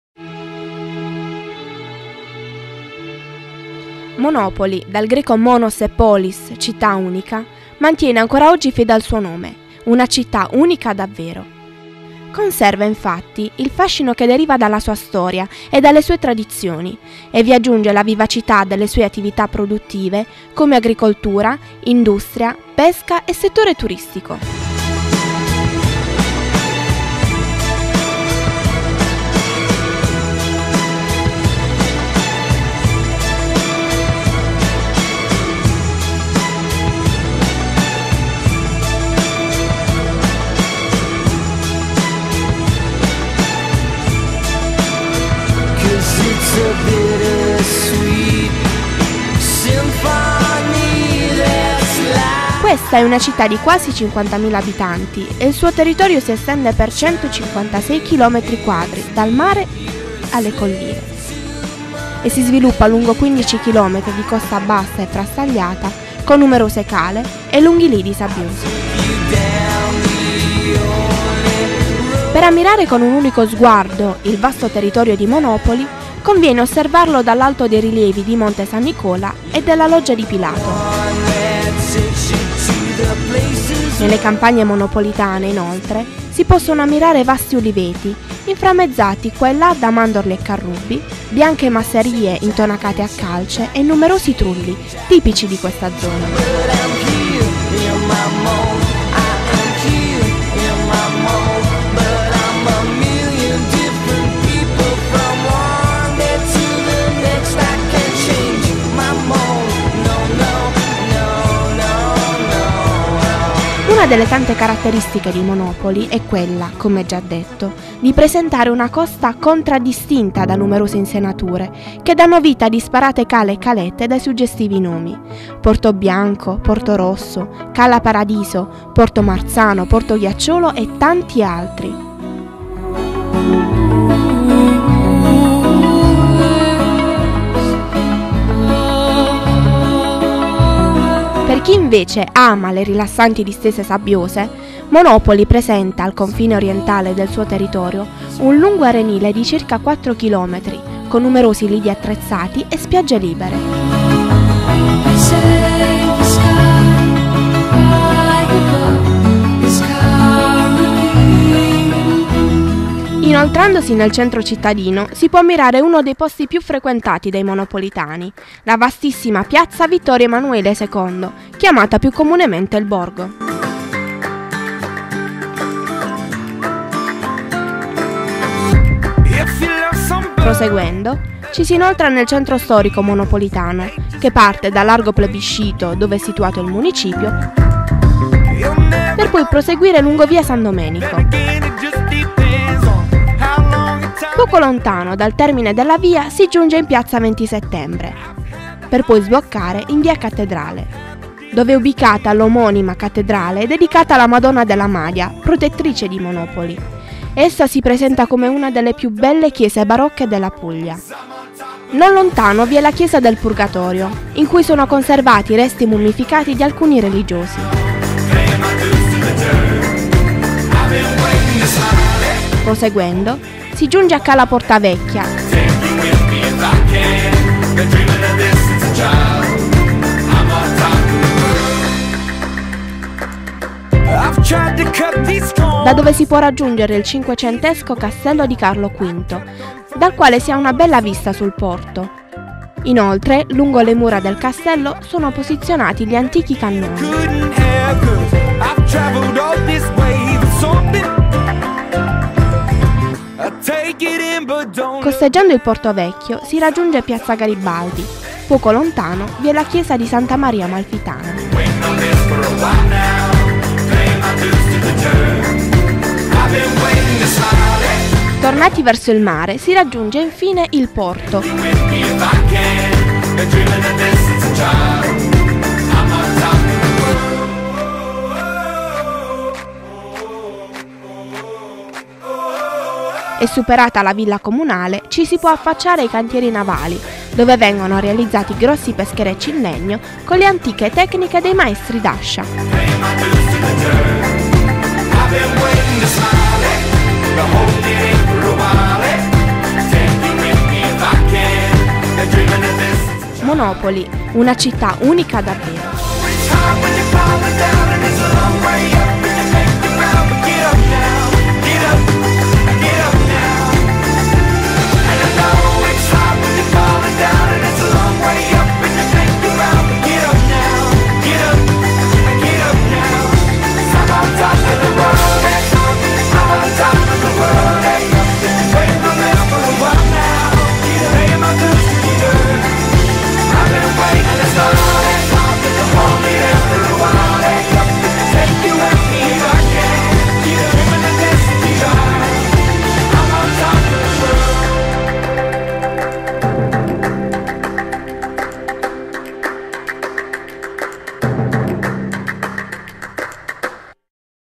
'Monopoli, città unica' ...un bel documentario per una bella città
Monopoli ...una città veramente unica !!! un documentario a cura del quartetto Altamura/Colucci/Minoia/Sportelli